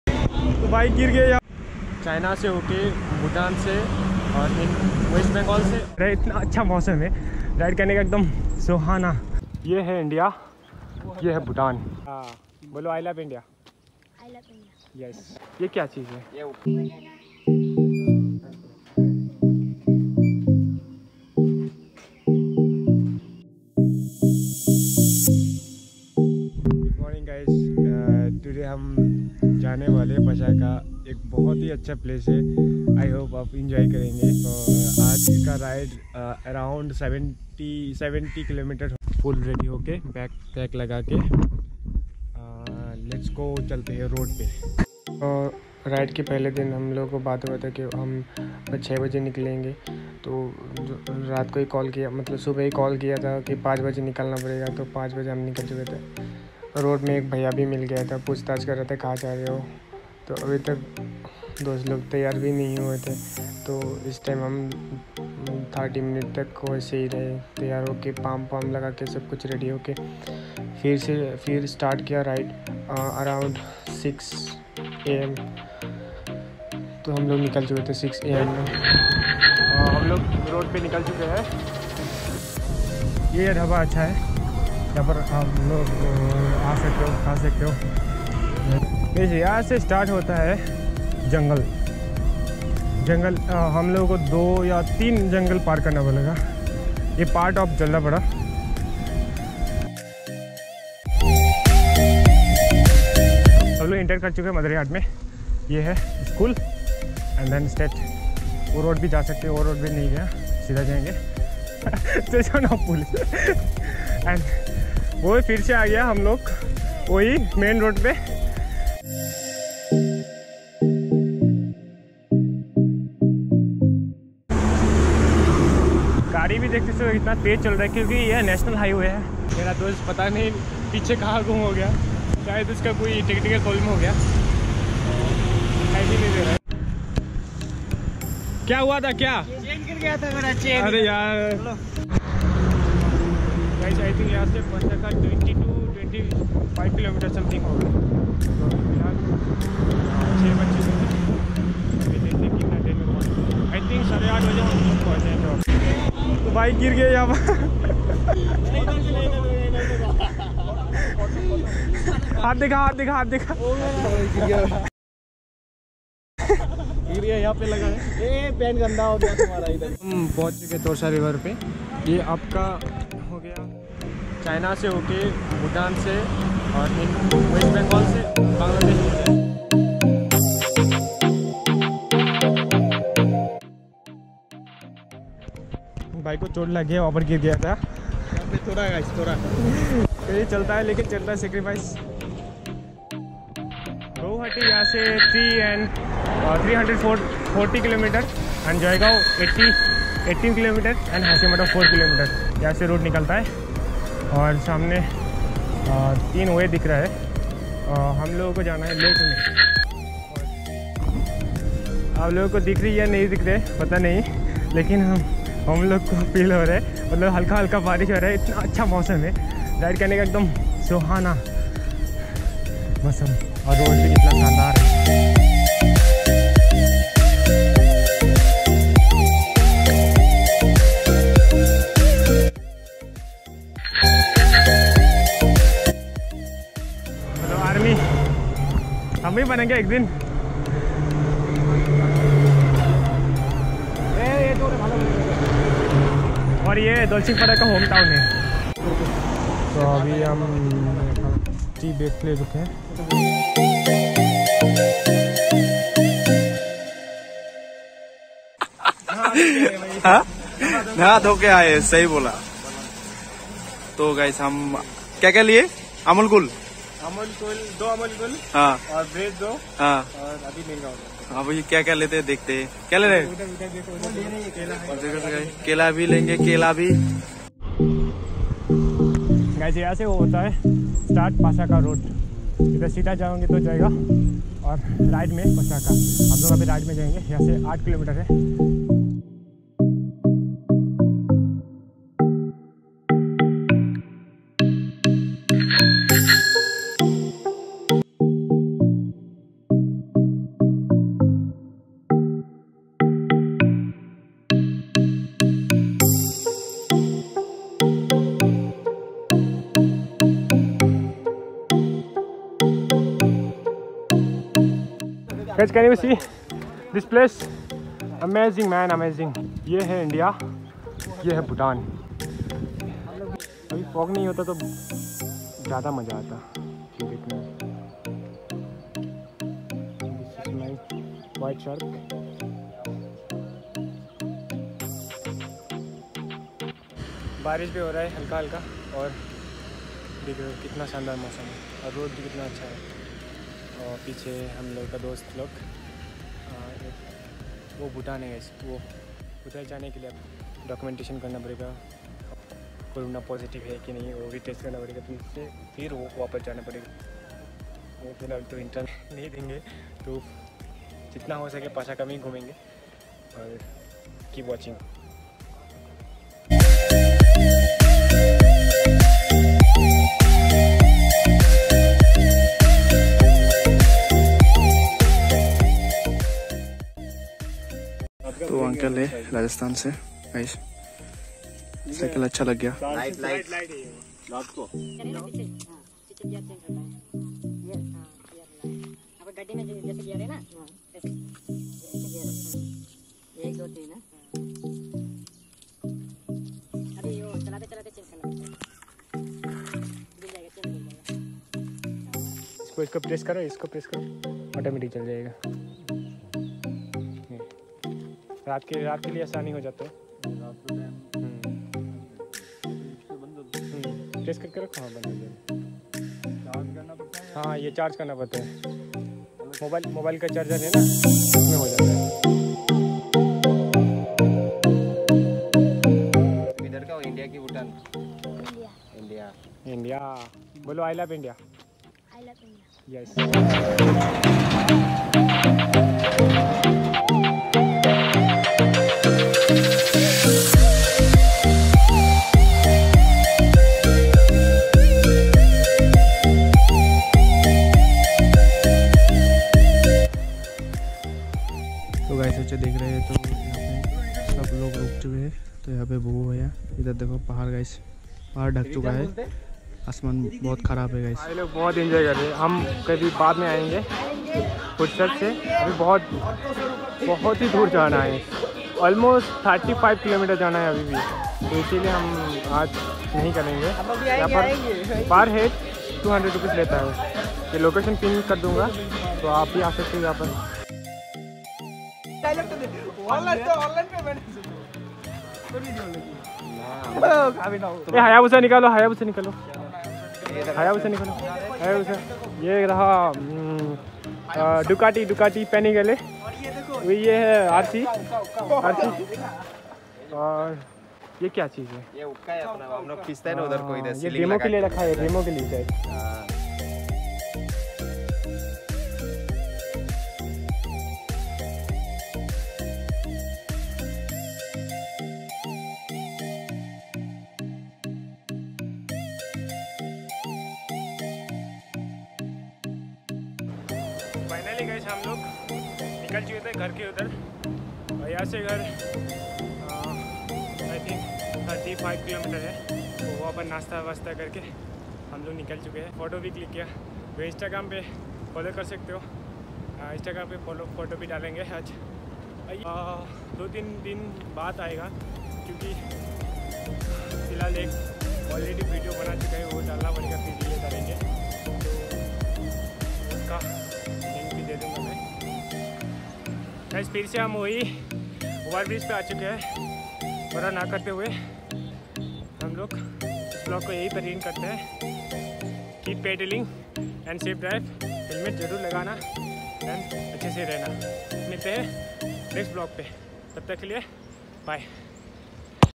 Cycle Ride India to Bhutan Borders | Pasakha | 80km cycle ride
Cycle Ride India to Bhutan Borders | Pasakha | 80km cycle ride Vlogs
wonderful place bhutan Borders Pasakha
West Bengle

LOVE FOR RONY BADARI

Ride state 6 am
My cycle :MTB
CAMRA : GoPro Hero 6

THANK YOU TO EVRYONE WATCHING THIS VIDEO